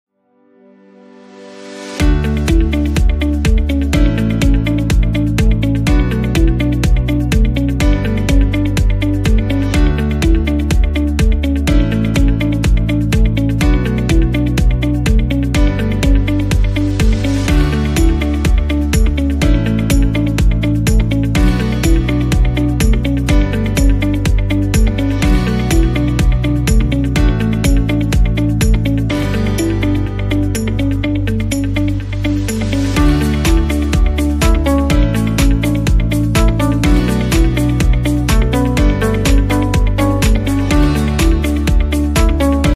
pcb 500kg/h Waste PCB recycling machine to separate precious metals from waste circuit boards
Do you want to process waste circuit boards to get precious metals? Waste PCB recycling machine is specially designed for doing that. It can separate precious metals from waste circuit boards with 99% separation rate, and without damaging the quality and environment.